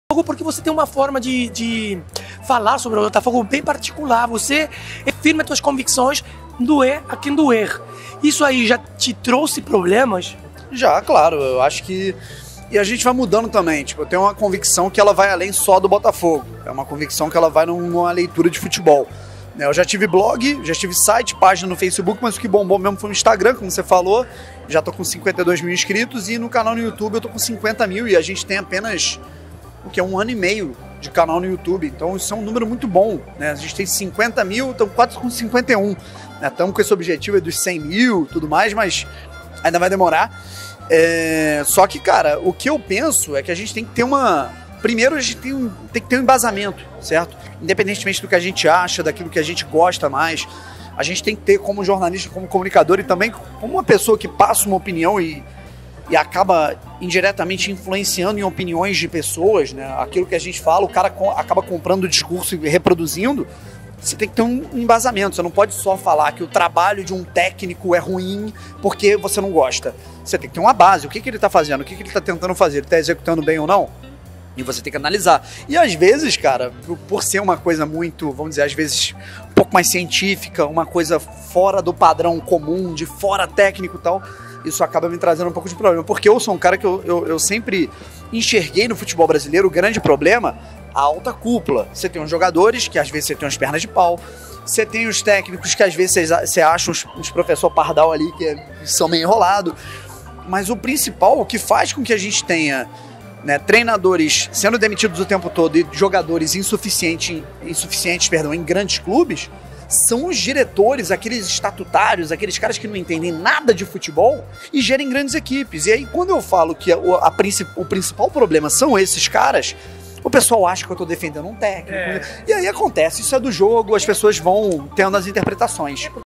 DANIEL BRAUNE JÁ TEVE PROBELMAS POR SER MUITO DIRETO NO SEUS COMENTARIOS? BRAUNEFOGO, DE VIRADA.
DAINEL BRAUNE NOS CONTA SE JÁ TEVE PROBLEMAS NOS SEUS COMENTARIOS COMO JORNALISTA.

ENTREVISTA DE VIRADA

CANAL BRAUNE FOGO:

BrauneFogo - Daniel Braune

DANIEL BRAUNE